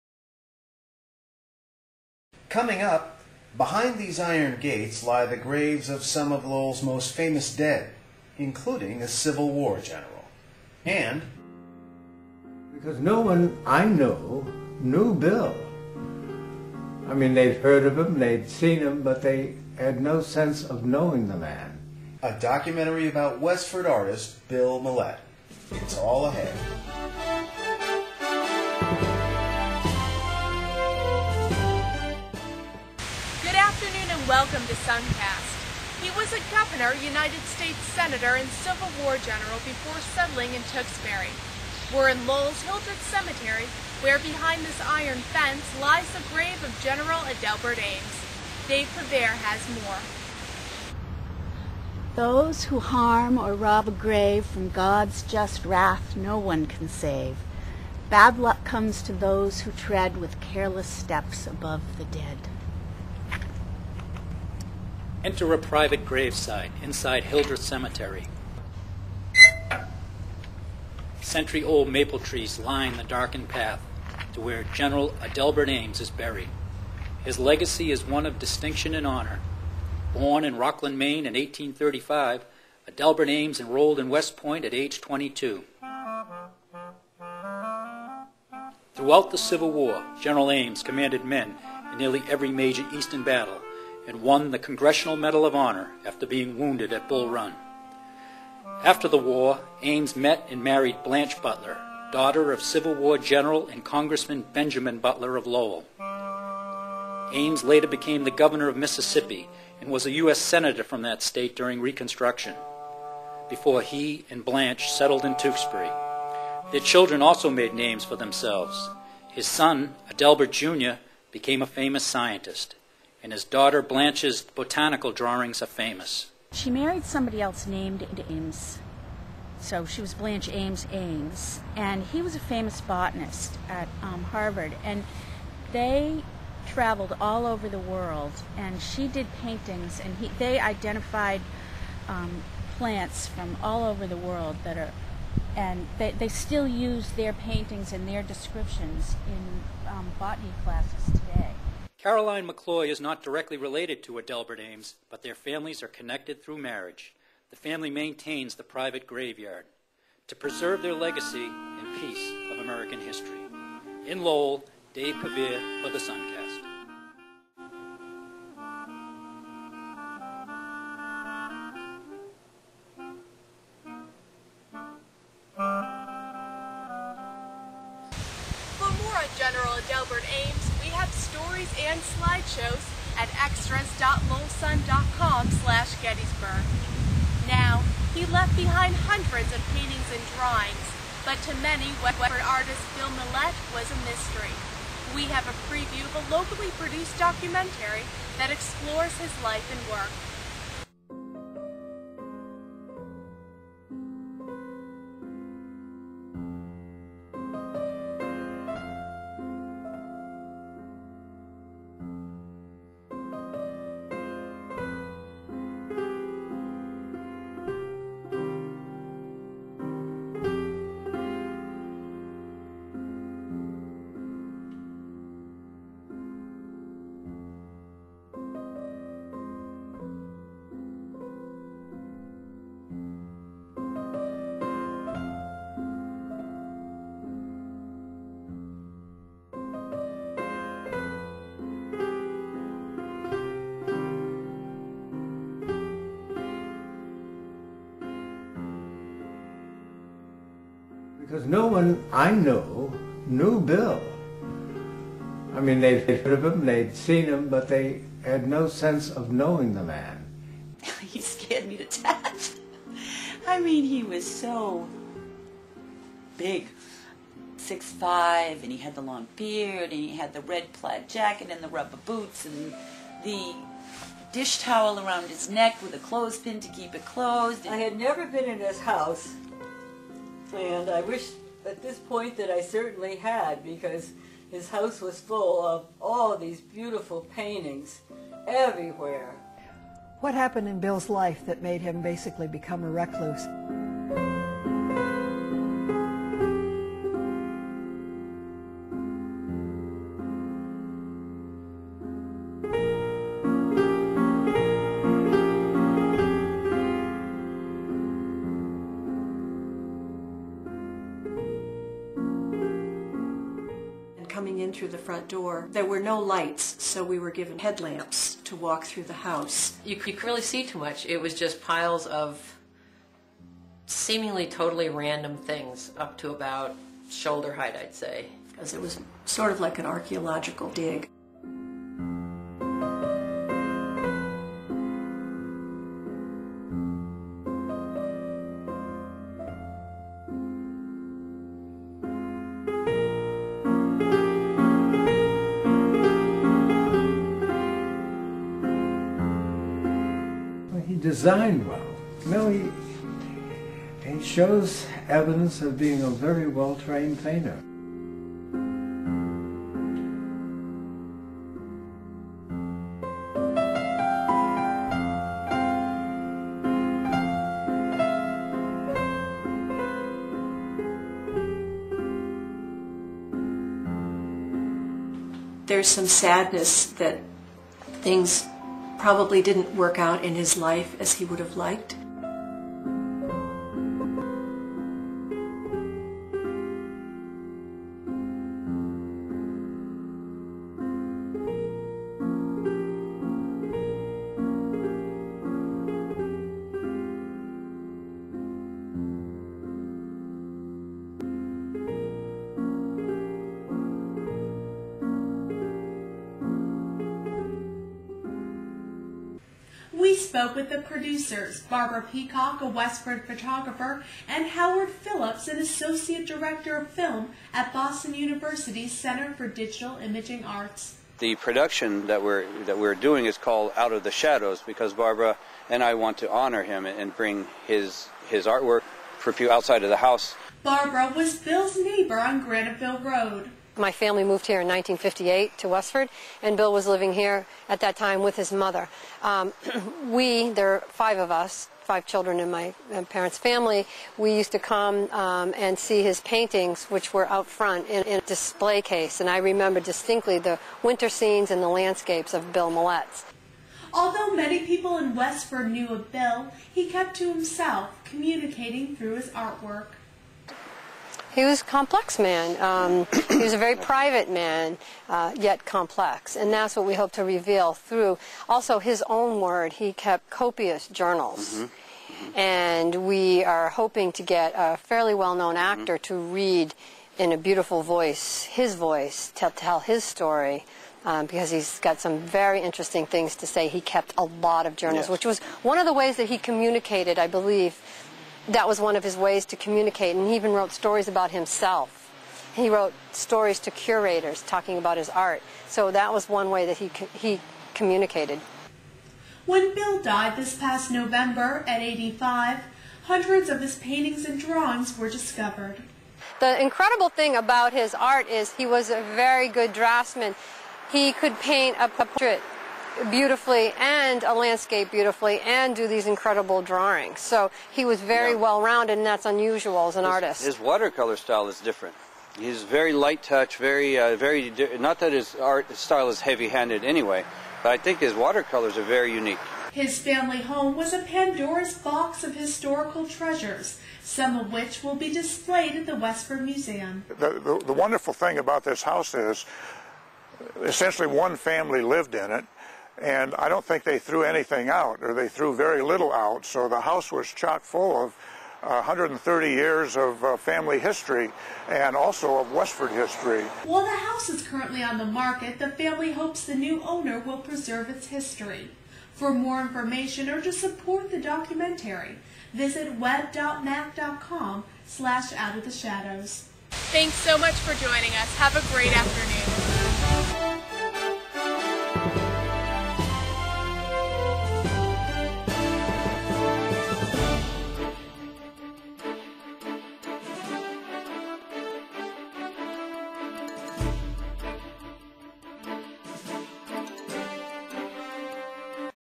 SunCast: Westford artist; Civil War
1. General Adelbert Ames, Gettysburg Hero
2. Legacy of a Westford artist